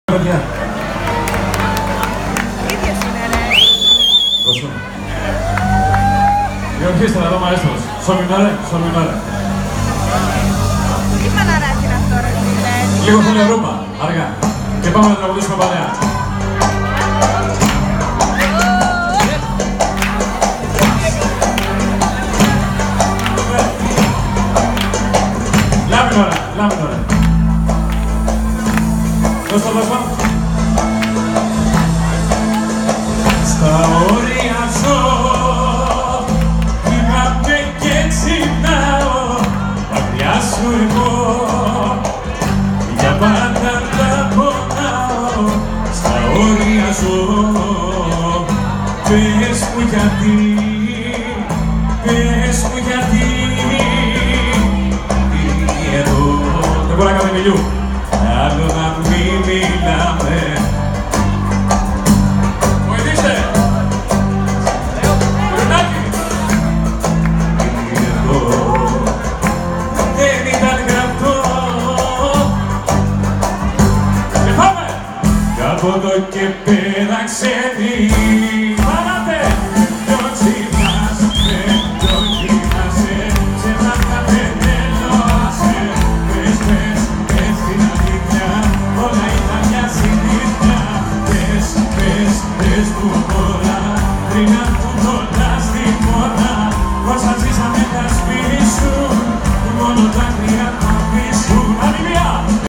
Petrelis live Autokinisi
Sto pleuro tou Stan stin Autokinisi o Thanos Petrelis dini to diko tou show